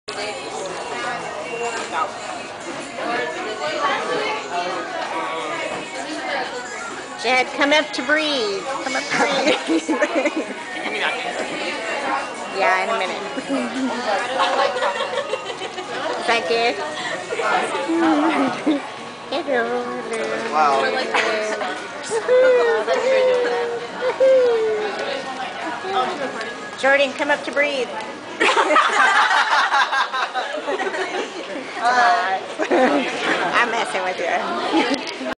Pitstop at MdD's with Quince Court
After practice at Hutchins, we stopped for a VERY quick bite at McD's.